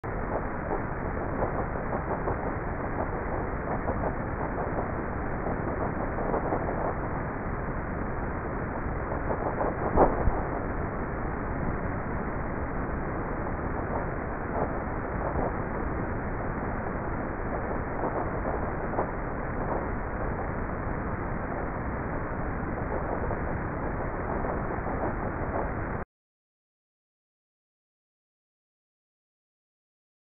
long term rat sna
Sympathetic nerve activity obtained in a conscious rat 70 days post telemetry implantation